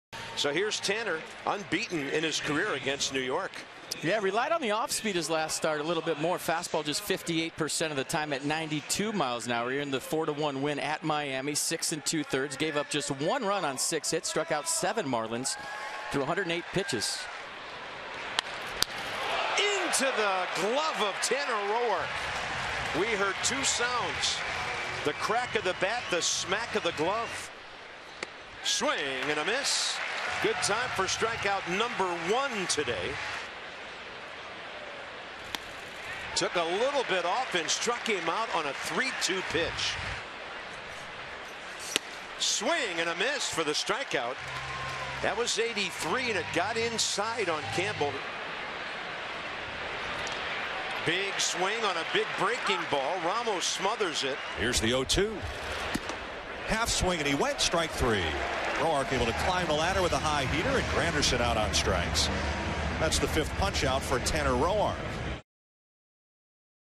NYM@WSH: Roark strikes out five over seven innings
Tanner Roark fans five batters over seven frames of two-run ball against the Mets